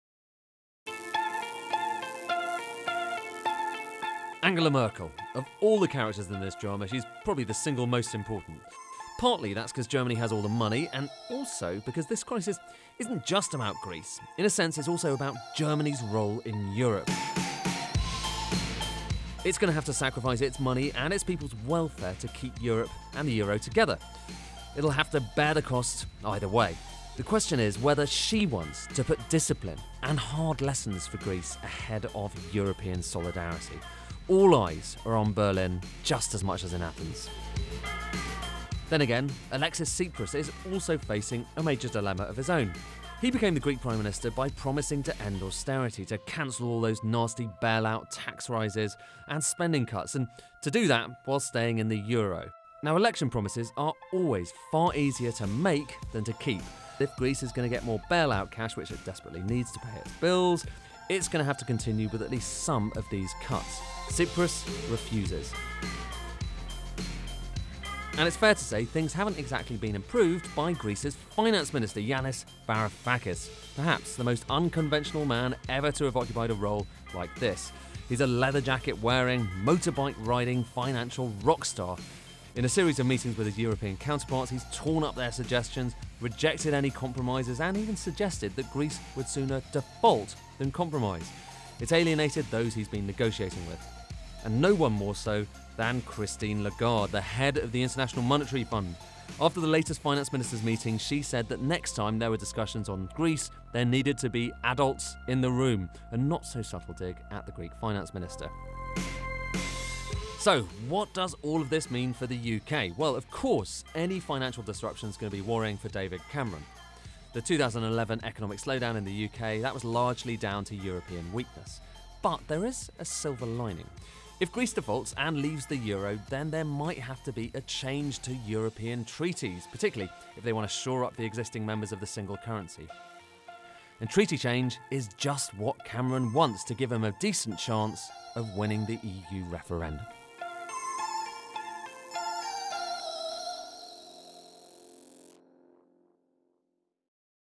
Using cartoons to explain the people behind the Eurozone crisis
With the help of a cartoonist, Sky's Ed Conway sketches out the five big players in the Grexit and understands the people behind the Eurozone crisis.

and download our apps